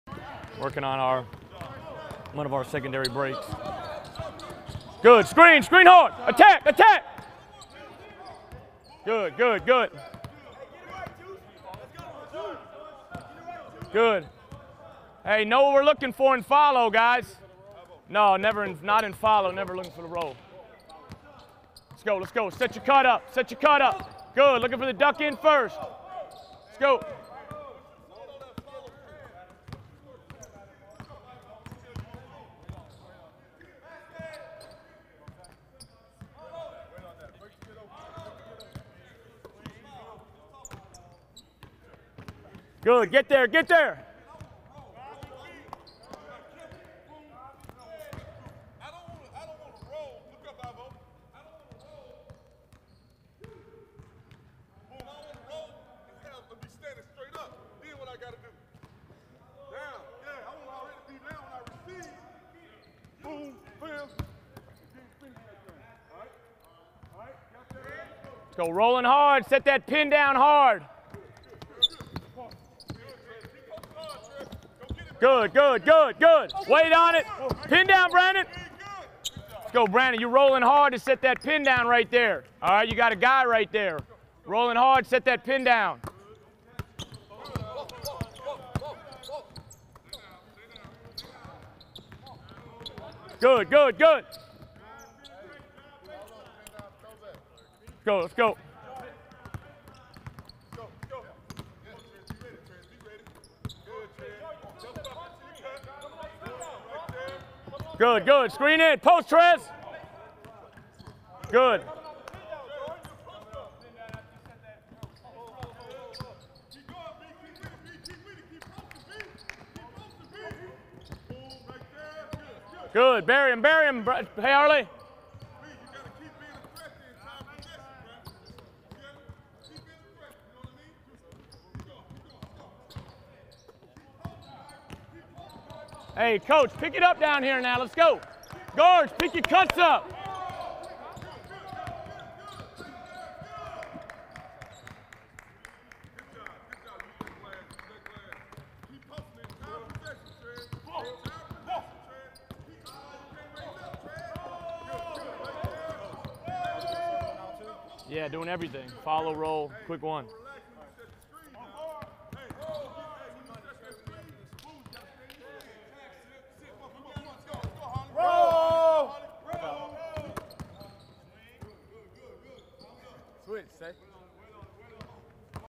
All Access Murray State University Basketball Practice
Steve Prohm is the former Murray State University Head Coach and led his team to the NCAA Tournament in his first year. This DVD takes you through multiple drills to breakdown their offensive and defensive system. Learn to build your offense through 5-on-0 play which will allow the repetition necessary for your team to have success.